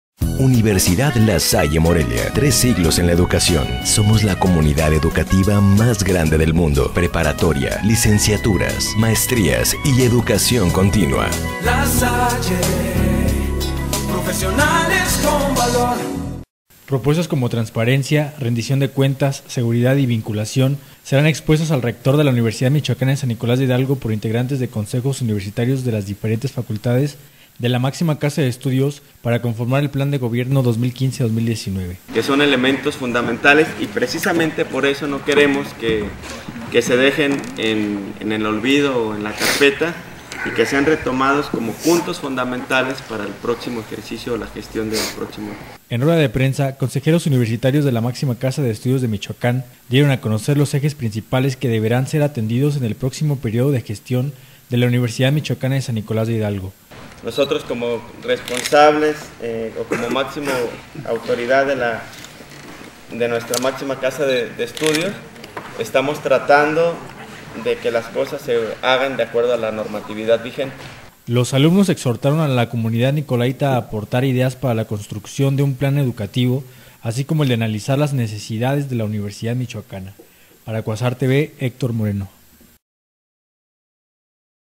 Proponen consejeros universitarios plan de gobierno 2015-2019 para la UMSNH
Propuestas como transparencia, rendición de cuentas, seguridad, vinculación, serán expuestas al rector de la Universidad Michoacana de San Nicolás de Hidalgo por integrantes consejeros universitarios de las diferentes facultades de la máxima casa de estudios para conformar el plan de gobierno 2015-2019